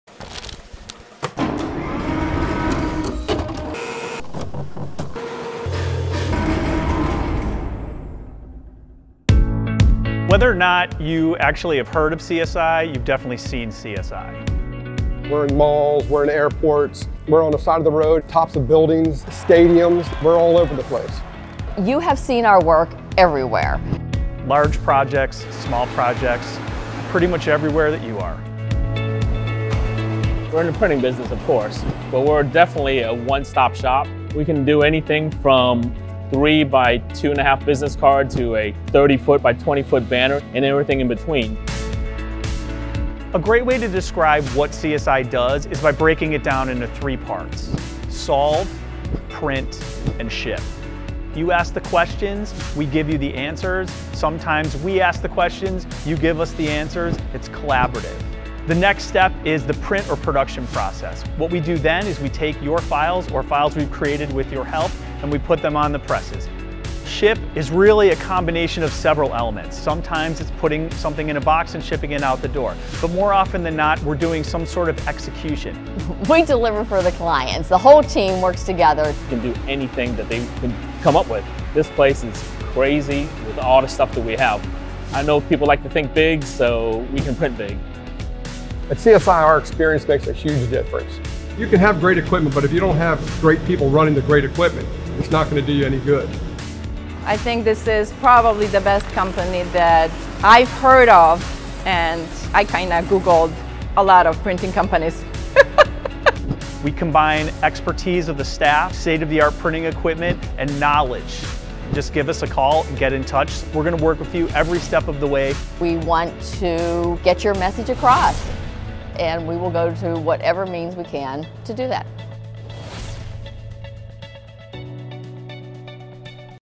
CSI Promo Video
A brief description of Composition Systems, Inc (CSI) and the services we provide. CSI is one of the leading print providers in the DC/MD/VA area and supplies all types of output to clients both nationally and internationally. Find out more by visiting csi2.com